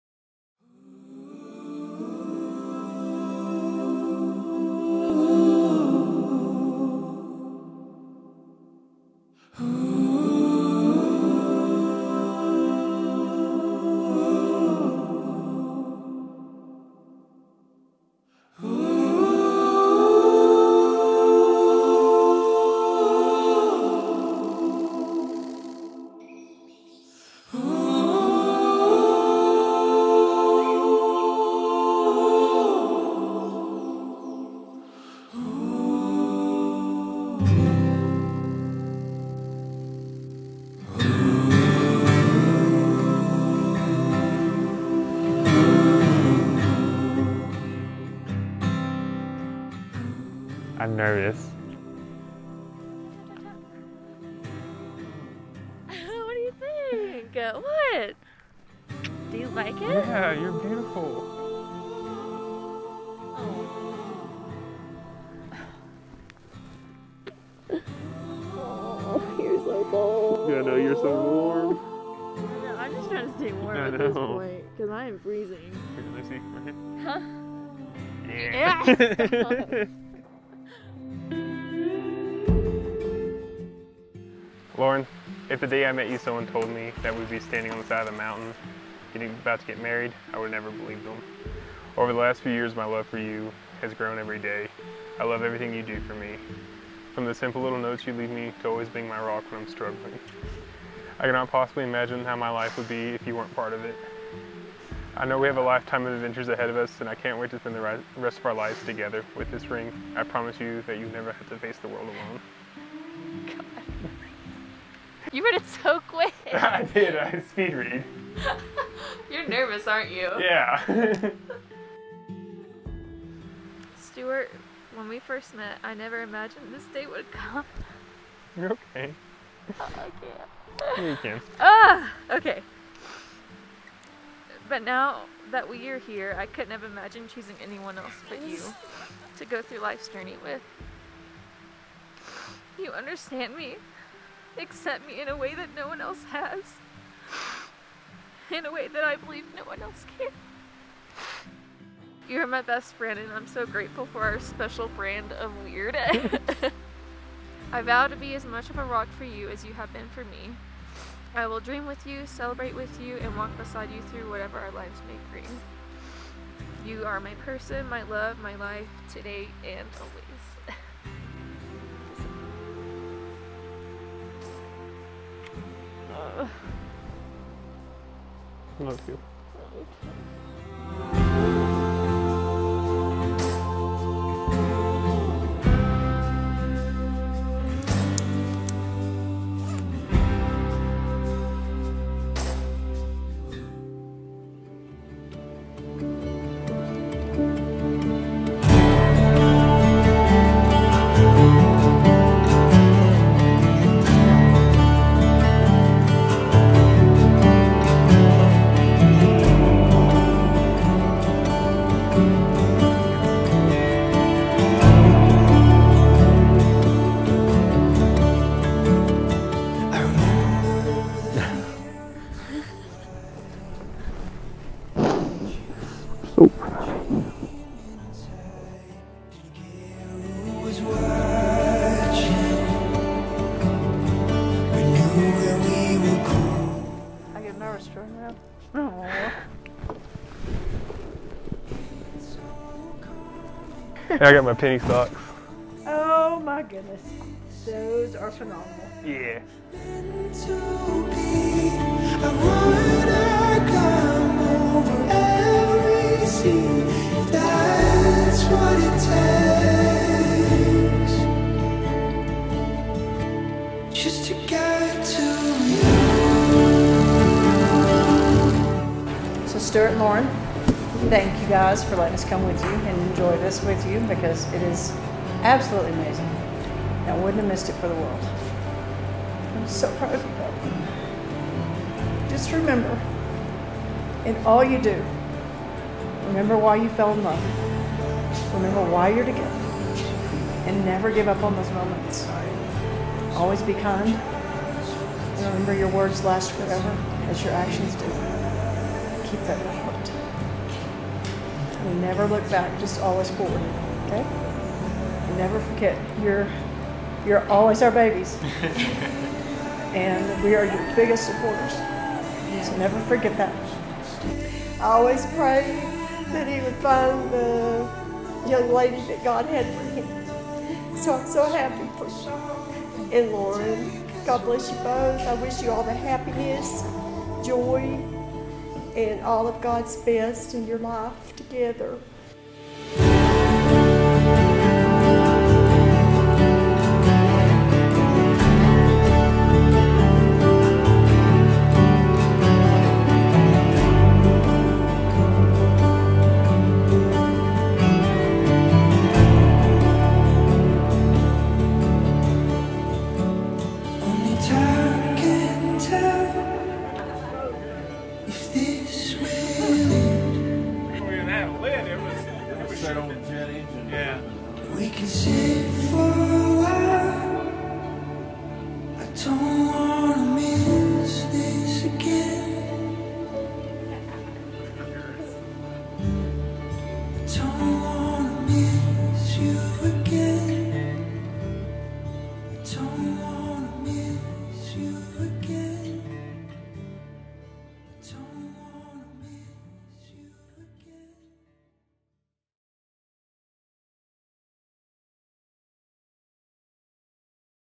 They said their vows at an alpine lake!
This was a 2 day elopement in the mountains of Colorado where the couple, the photographer and I hiked up a mountain at sunrise to exchange vows at a blue alpine lake. The following day, the couple got ready in an a-frame cabin in the woods where they had a first look with their parents followed by a ceremony on the deck. The day ended with us getting shots in the colorful fall trees and a bonfire with smore's! They brought along their blonde weiner dog, Penny, and had their best friend officiate. It was overall a very adventure packed yet relaxed elopement filled with tears, laughter and nature!